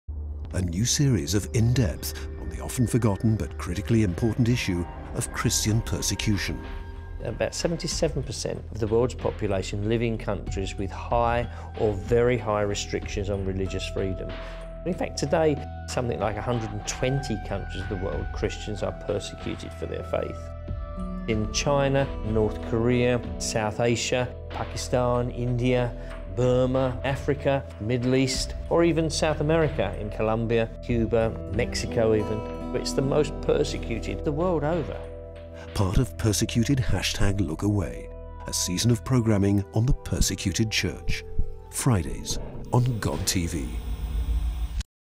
In Depth: Persecuted Church
Gain new perspective on the harsh reality of believers being persecuted for their faith around the world. Don’t miss this interview with Mervyn Thomas who heads up Christian Solidarity Worldwide, an organisation working for religious freedom for all. Gain new perspective on the immense cost of following Christ in some countries and choose to not LookAway.